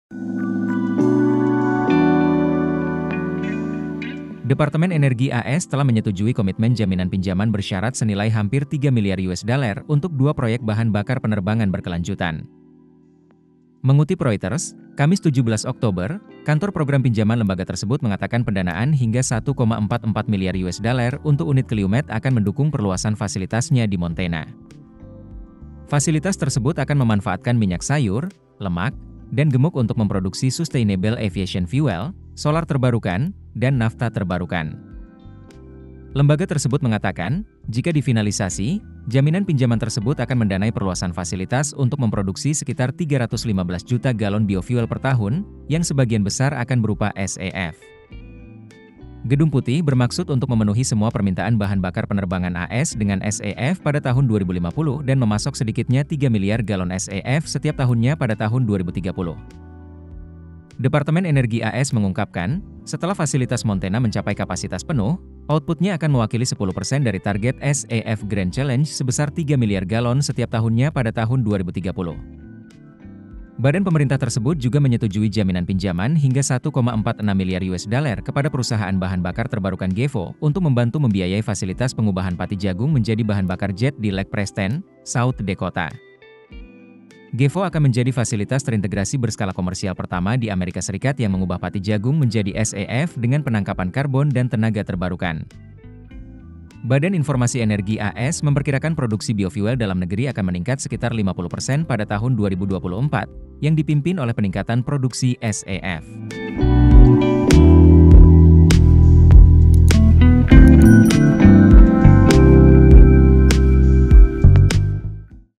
AS Beri Jaminan Pinjaman US$ 3 Miliar ke Proyek Bahan Bakar Penerbangan Berkelanjutan | KONTAN News
Departemen Energi AS telah menyetujui komitmen jaminan pinjaman bersyarat senilai hampir US$ 3 miliar untuk dua proyek bahan bakar penerbangan berkelanjutan.

Mengutip Reuters, Kamis (17/10), Kantor Program Pinjaman lembaga tersebut mengatakan pendanaan hingga US$ 1,44 miliar untuk unit Calumet akan mendukung perluasan fasilitasnya di Montana.

Fasilitas tersebut akan memanfaatkan minyak sayur, lemak, dan gemuk untuk memproduksi sustainable aviation fuel (SAF), solar terbarukan, dan nafta terbarukan.

Lembaga tersebut mengatakan, jika difinalisasi, jaminan pinjaman tersebut akan mendanai perluasan fasilitas untuk memproduksi sekitar 315 juta galon biofuel per tahun, yang sebagian besar akan berupa SAF.

Gedung Putih bermaksud untuk memenuhi semua permintaan bahan bakar penerbangan AS dengan SAF pada tahun 2050 dan memasok sedikitnya 3 miliar galon SAF setiap tahunnya pada tahun 2030.

Departemen Energi AS mengungkapkan, setelah fasilitas Montana mencapai kapasitas penuh, outputnya akan mewakili 10% dari target SAF Grand Challenge sebesar 3 miliar galon setiap tahunnya pada tahun 2030.

Badan pemerintah tersebut juga menyetujui jaminan pinjaman hingga US$ 1,46 miliar kepada perusahaan bahan bakar terbarukan Gevo untuk membantu membiayai fasilitas pengubahan pati jagung menjadi bahan bakar jet di Lake Preston, South Dakota.

Gevo akan menjadi fasilitas terintegrasi berskala komersial pertama di Amerika Serikat yang mengubah pati jagung menjadi SAF dengan penangkapan karbon dan tenaga terbarukan.

Badan Informasi Energi AS memperkirakan produksi biofuel dalam negeri akan meningkat sekitar 50% pada tahun 2024, yang dipimpin oleh peningkatan produksi SAF.